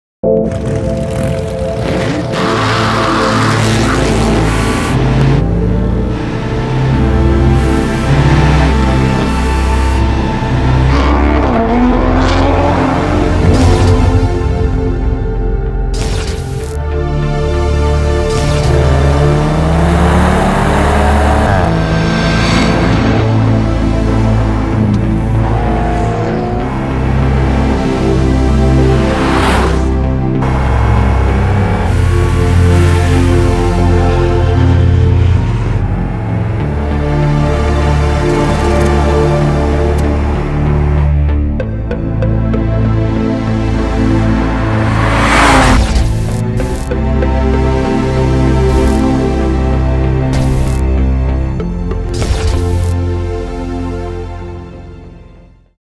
Volkswagen Scirocco R on the race track ENG YouTube
Experience driving sequences of the Scirocco R on the race track.